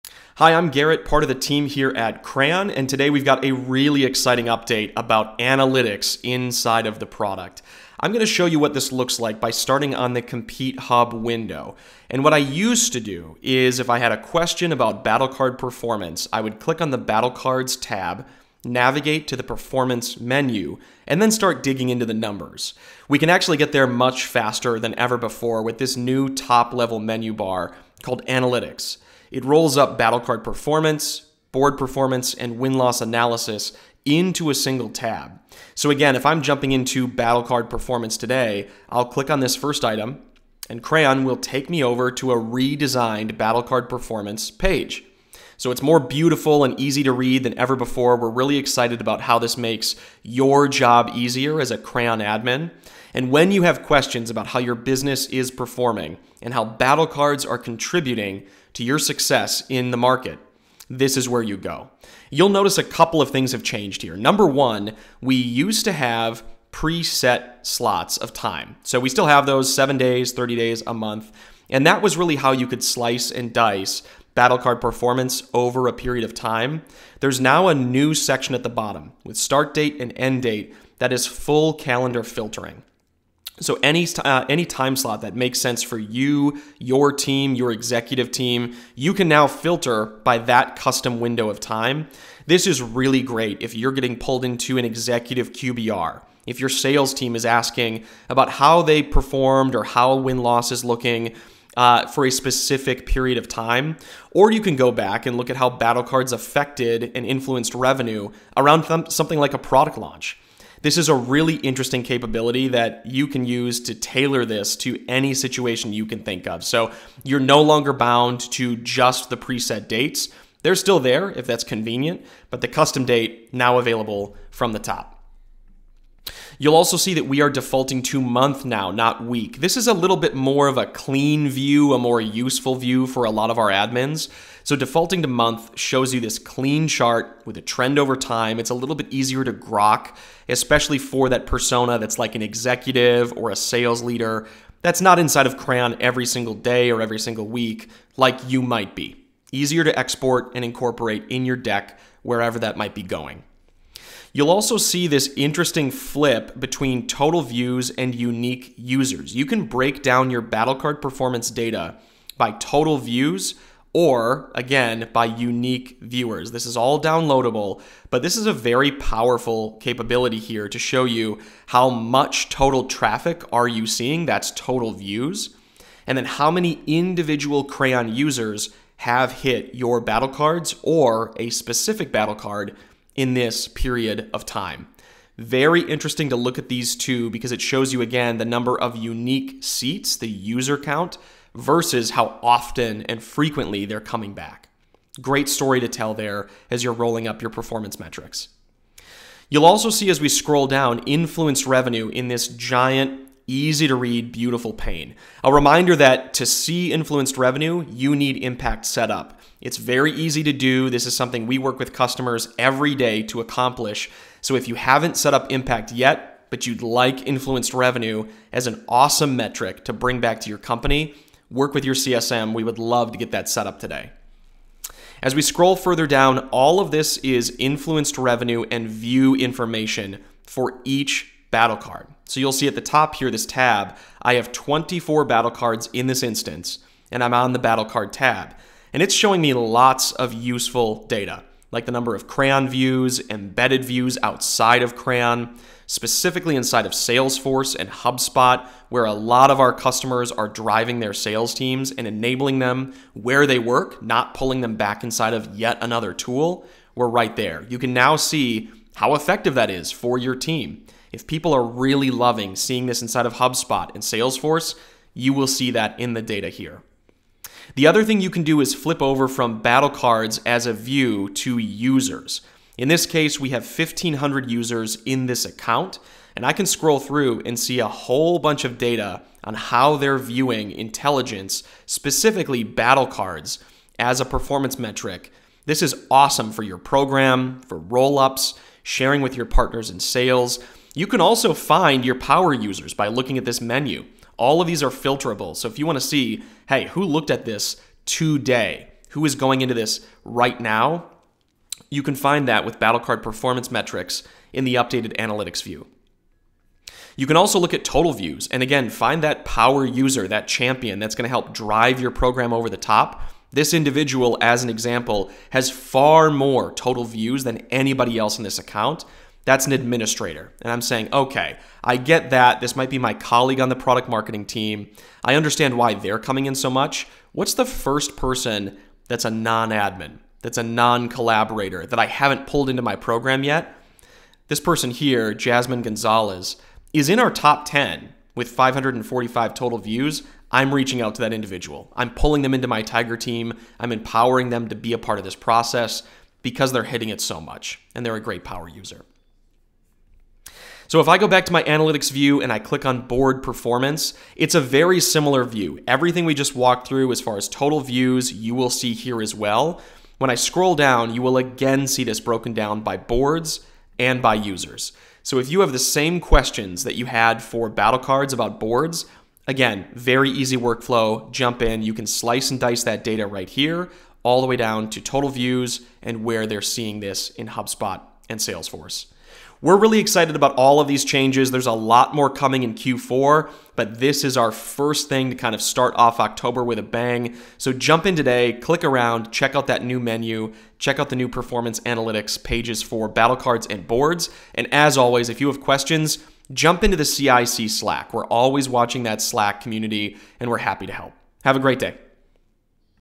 Crayon Battlecards and Boards Analytics Tutorial
This quick tutorial provides an overview of Crayon's updated analytics feature for measuring battlecard and board performance.